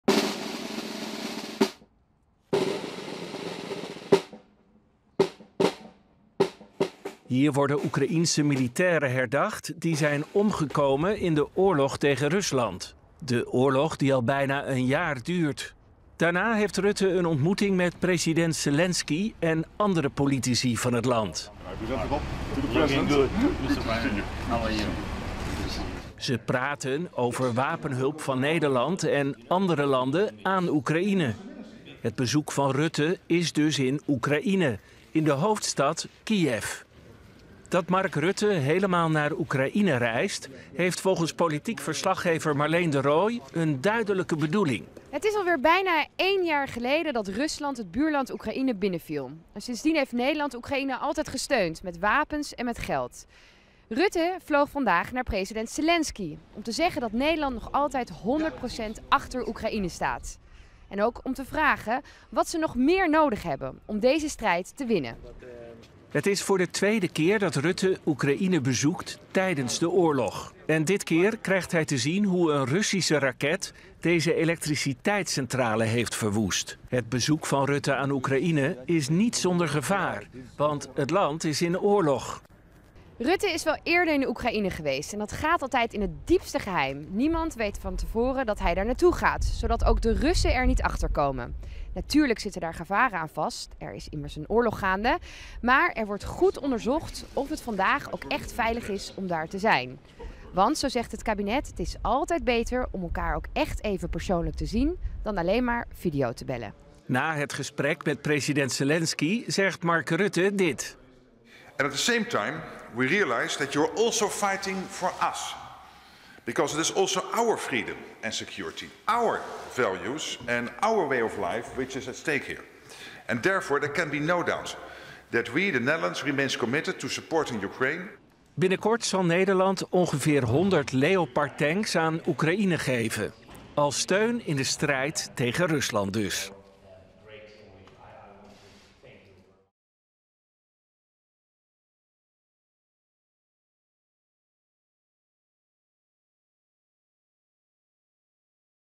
Premier Rutte bezoekt Oekraïne: 'We blijven jullie steunen'
Premier Rutte was vandaag op bezoek bij president Zelensky van Oekraïne. Ze praten over wapenhulp van Nederland en andere landen aan Oekraïne.

Rutte heeft gezegd dat Nederland steun blijft geven aan Oekraïne. Zo werd eerder bekend dat het land ongeveer honderd Leopard-tanks krijgt van Nederland. Die zijn belangrijk in de strijd tegen Rusland.